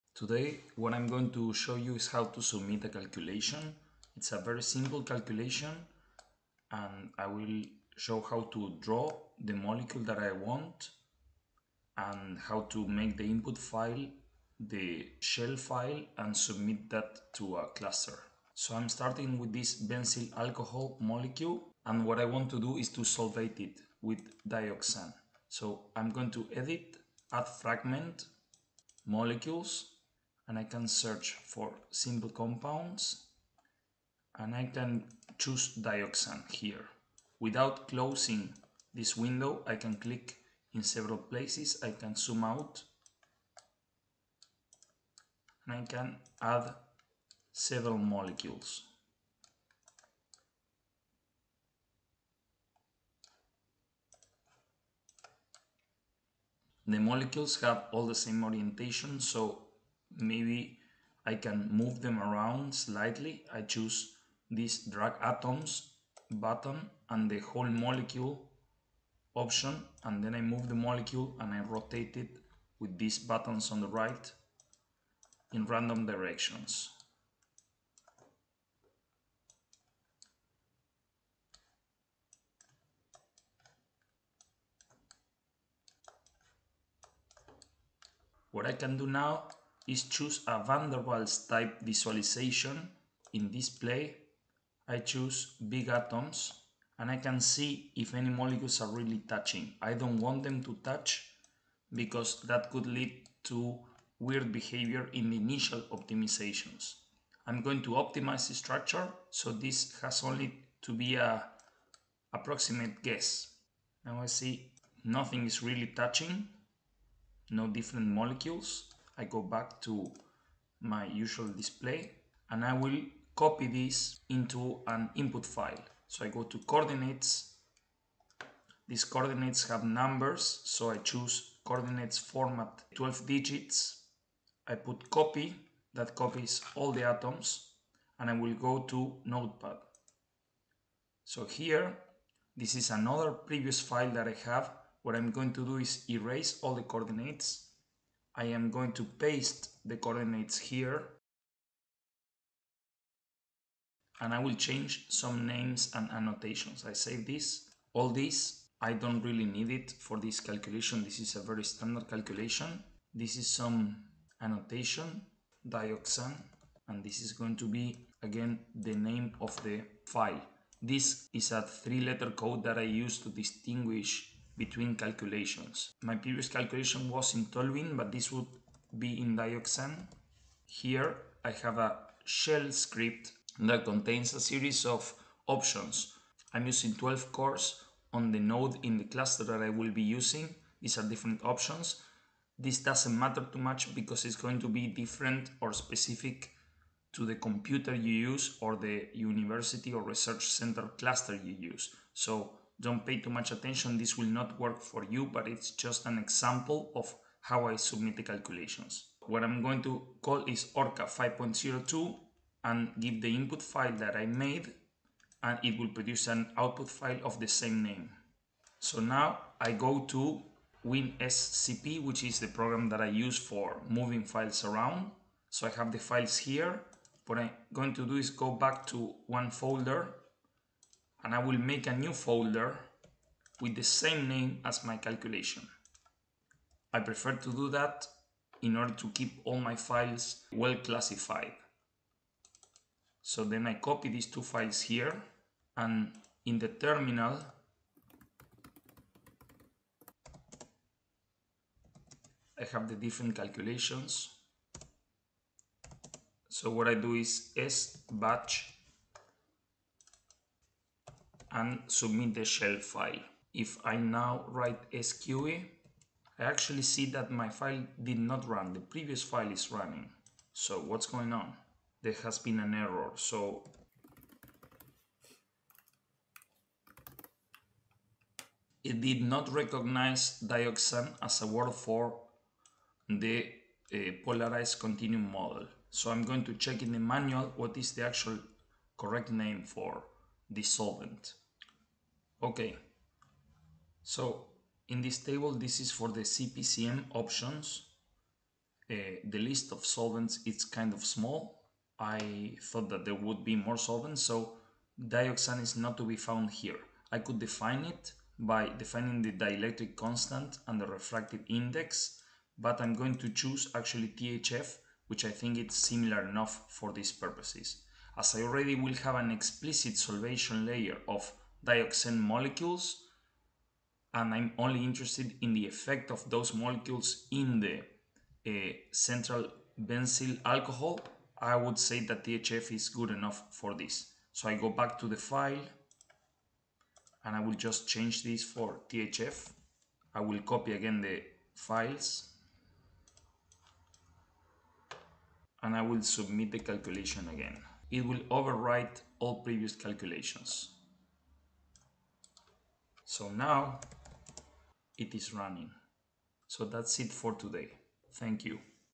How to Run a Geometry Optimization in Orca 5.0 - Start to Finish Workflow
I show how to create an input structure for a solvated molecule in Chemcraft, then how to include that structure in an Orca input file. I also show the BASH scripts I use, and then submit the calculation in a cluster.
The details of the methods can be found in many of my previous videos. The purpose was to show the complete workflow.